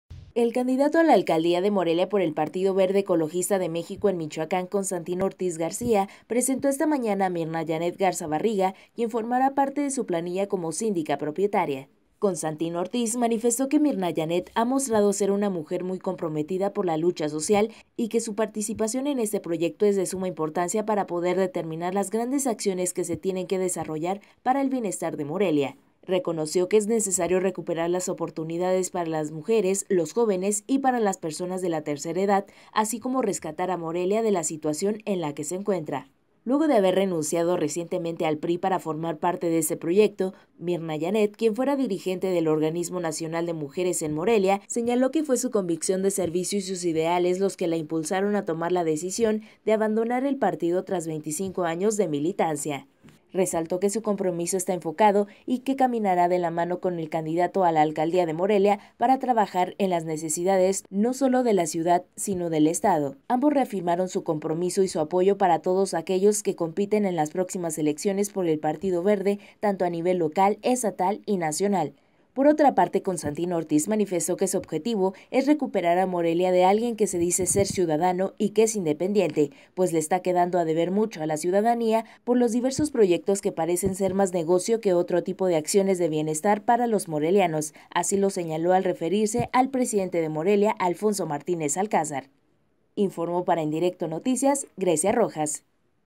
Constantino Ortiz
La ex dirigente de mujeres priístas en Morelia va de síndico con Constantino Ortíz, deja el partido tras 25 años de militancia.